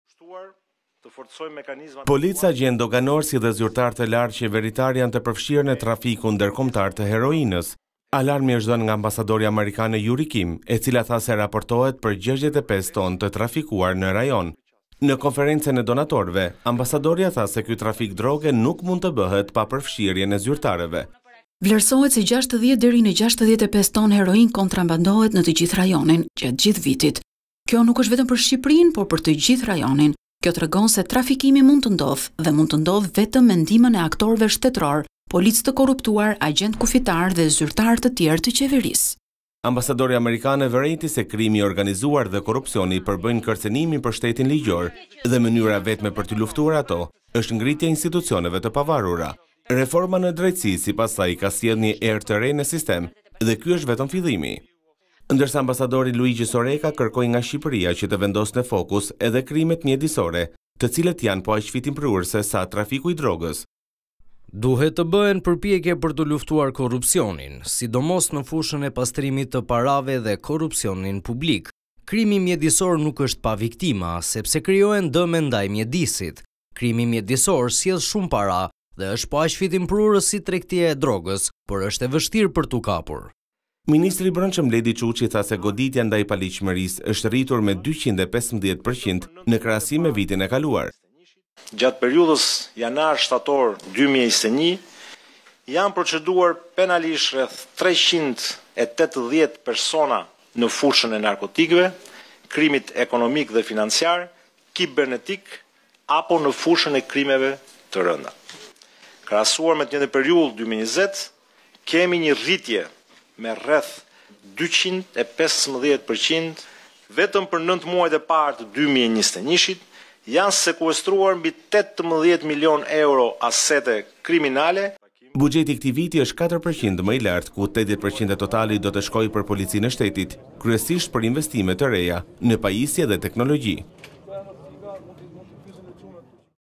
Arrestimi i politikanëve të korruptuar/ Paralajmëron Kim: Ky është vetëm fillimi! - Vizion Plus
Ambasadorja e SHBA në Shqipëri, Yuri Kim ka marrë pjesë sot në një konferecë, ku në fokus është lufta kundër krimit, pastrimit të parave dhe krimit kibernetik Kim theksoi se se largimi i gjyqtarëve dhe prokurorëve dhe pastrimi i politikës nga elementët e korruptuar është vetëm fillimi. Ambasadroja tha se e vetmja mënyrë për të luftuar krimin e organizuar është që të kemi institucione të pavarura që mbështesin zbatimin e ligjit. “Kur njerëzit e ndershëm shohin politikanë të korruptuar ata humbasin besimin në sistem. Atyre duhet t’u garantohet se do të kenë çdo shans përballë njerëzve të tillë. E vetmja mënyrë për të luftuar krimin e organizuar është të kemi institucione të pavarura që mbështesin zbatimin e ligjit.Shqipëria ka arritur suksese të vërteta në vitin 2021 që nga largimi i gjyqtarëve dhe prokurorëve të korruptuar, arrestimi i politikanëve. Ky është vetëm fillimi. Ne po shohim një epokë të re. Ekziston ndjesia se pandëshkueshmëria po merr fund. 60-65 ton heroinë kontrabandohen në rajon gjatë vitit. Kjo tregon që trafikimi mund të ndodhë vetëm me ndihmën e aktorëve shtetërorë. Trafikimi i qenieve njerëzore është rritur gjithashtu. Mijëra emigrantë kalojnë përmes Shqipërisë për në BE. Korrupsioni luan një rol në këtë gjë. Grupe e krimit të organizuar vazhdojnë të pastrojnë paratë. Ajo për të cilën jemi për të folur këtu sot është se si ministria e Brendshme mund ta bëjë Shqipërinë një vend më të mirë për të gjithë. Duhet të rritet transparenca dhe llogaridhënia”, tha Yuri Kim.